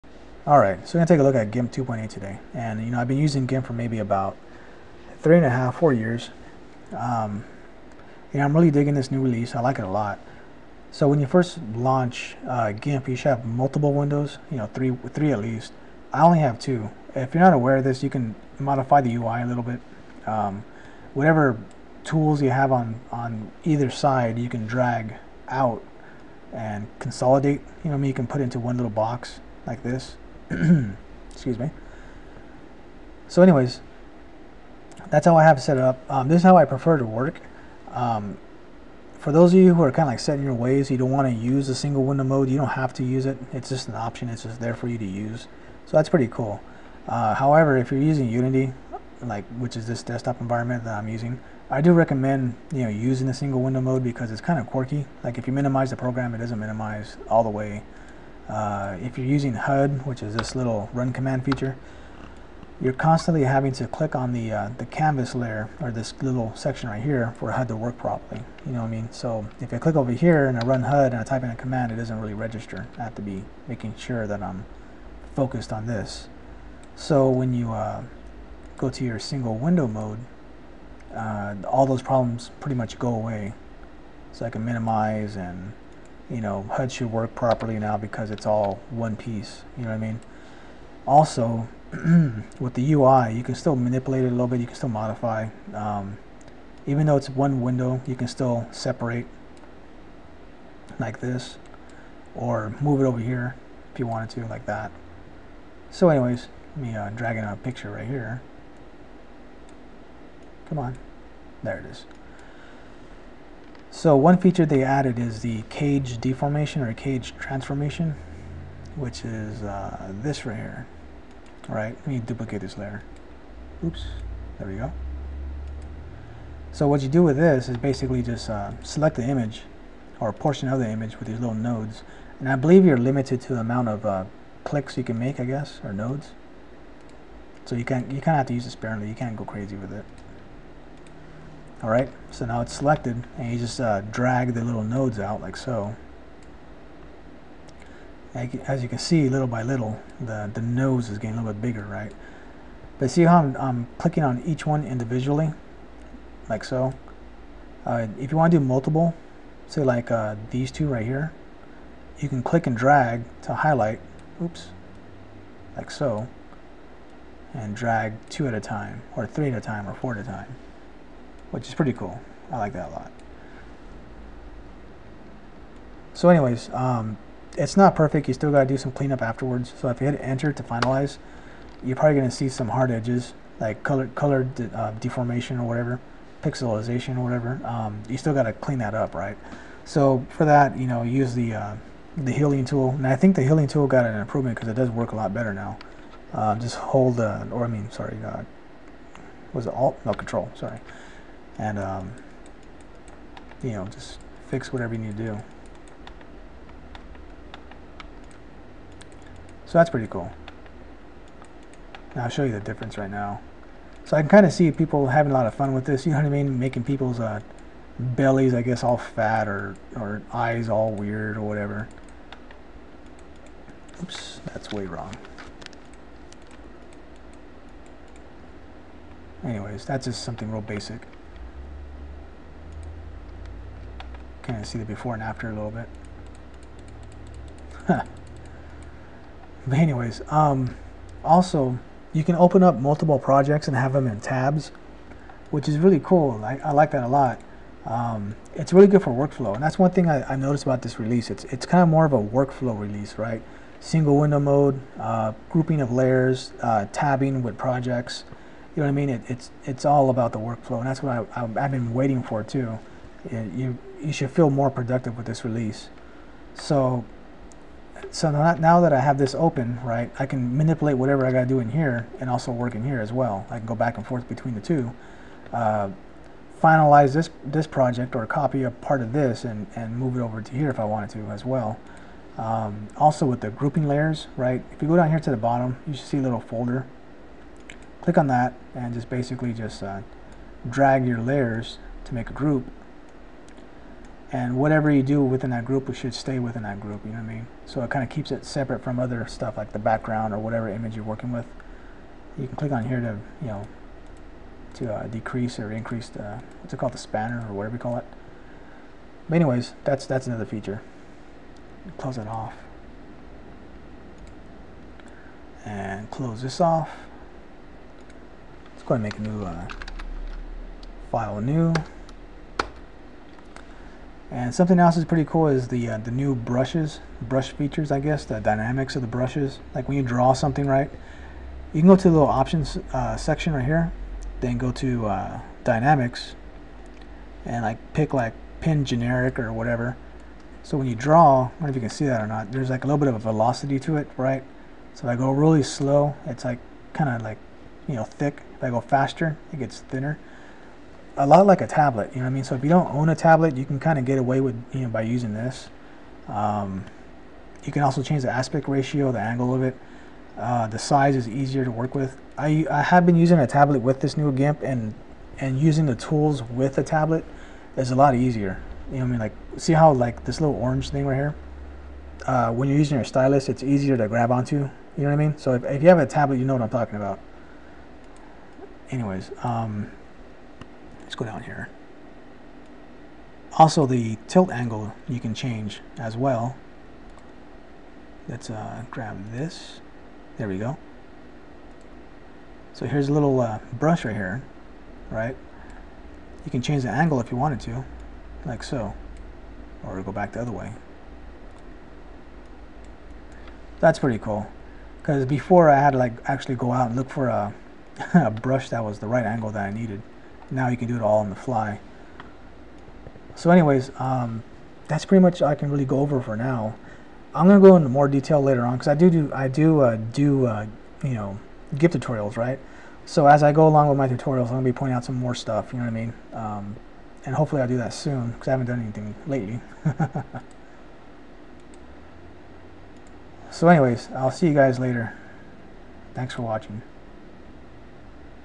gimp 2.8 ubuntu 12.04
to install
sudo add-apt-repository ppa:otto-kesselgulasch/gimp
sudo apt-get update && sudo apt-get install gimp

the plugin registry should work now but you must remove the gimp-resynthesizer.